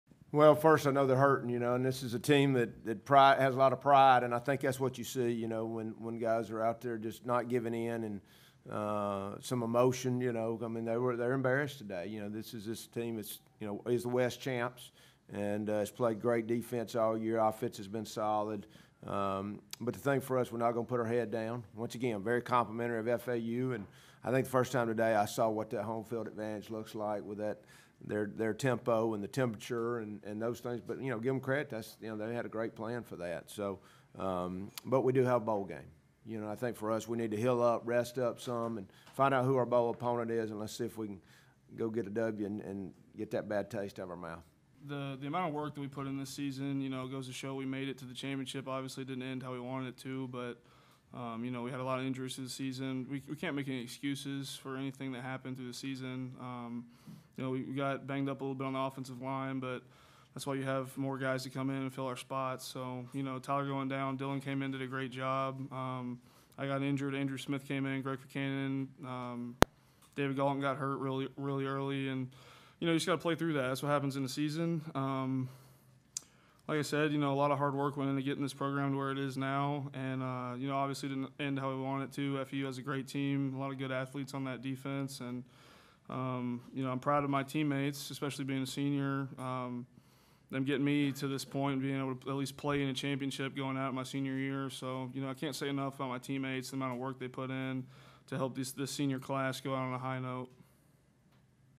UAB coach Bill Clark on C-USA title game loss at FAU
Kiffin, FAU roll past UAB 49-6 for Conference USA title